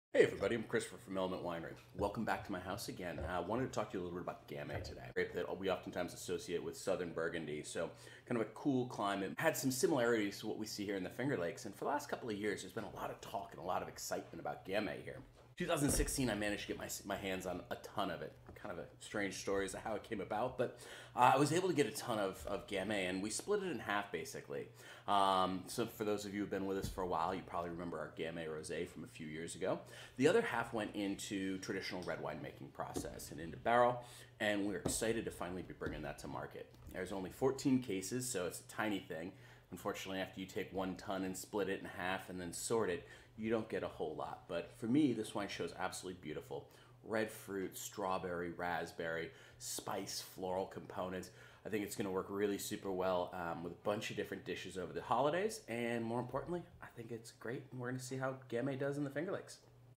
Gamay
A little intro to our Gamay, a beautiful wine not usually seen in the Finger Lakes. We made only 14 cases.